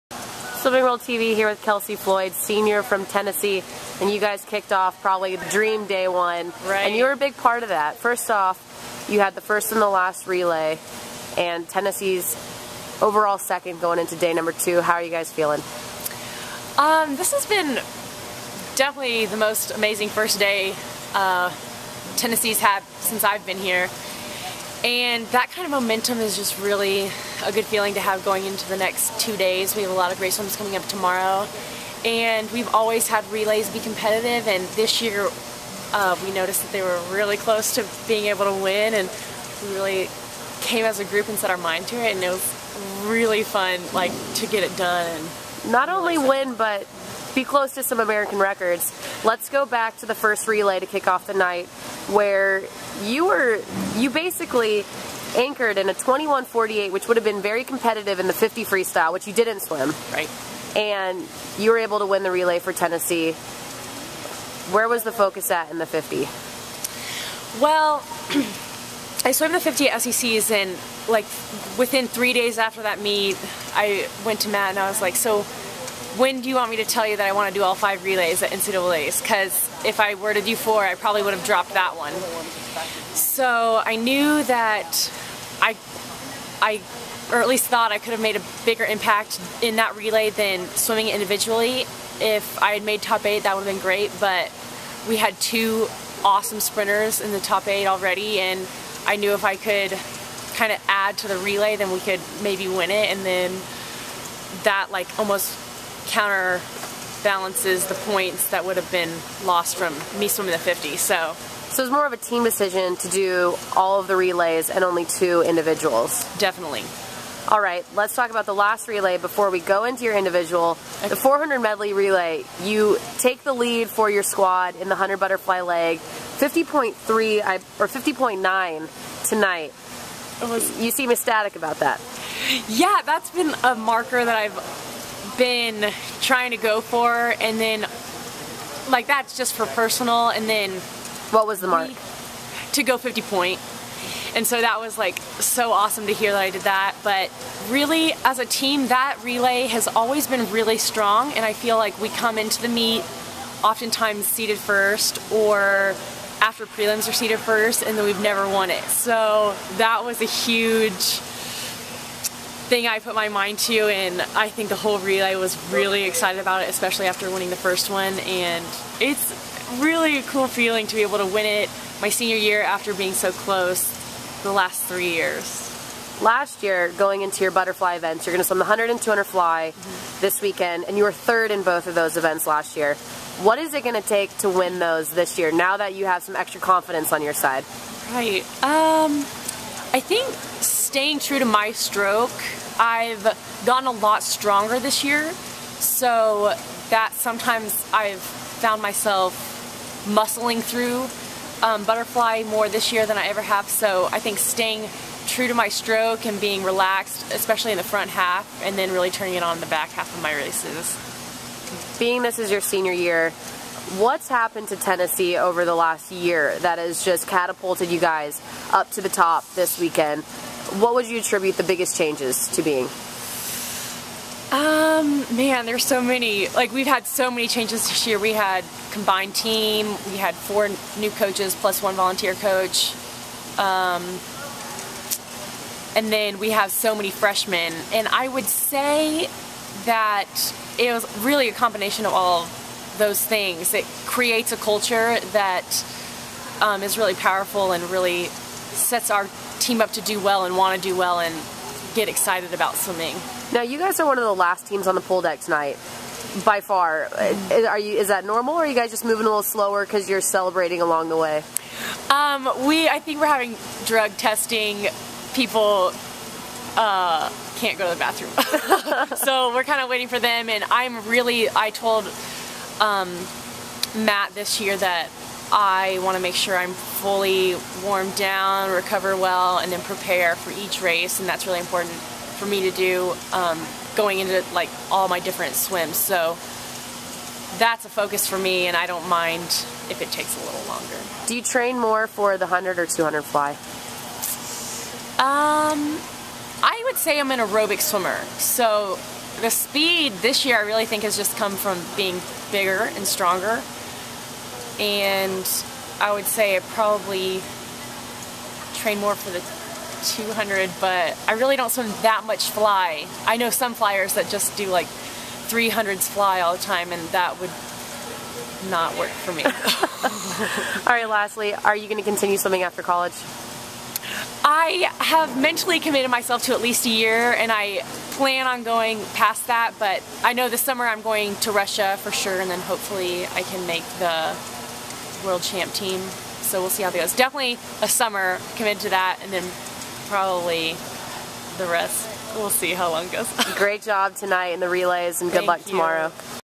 Kelsey Floyd (Tennessee)
Tennessee's Kelsey Floyd talks with SwimmingWorld.TV after her epic night that netted a pair of relay titles for the Lady Vols.